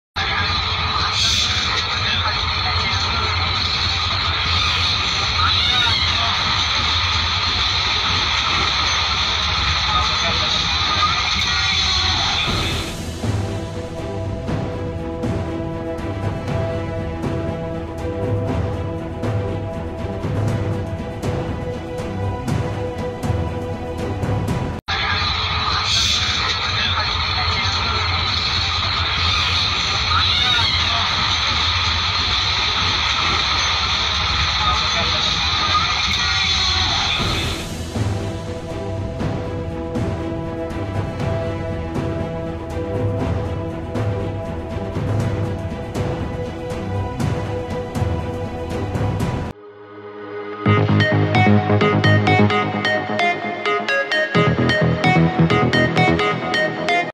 ଏମିତି ହୋଇଥିଲା ବାହାନଗା ଦୁର୍ଘଟଣା | Odisha Reporter
ସାମ୍ନାକୁ ଆସିଲା ବାହାନଗା ଦୁର୍ଘଟଣା ସମୟର ଭିଡିଓ । ସେସିଆଲ ମିଡିଆରେ ମୋବାଇଲରୁ ମୋବାଇଲ ଘୂରି ବୁଲୁଛି  ।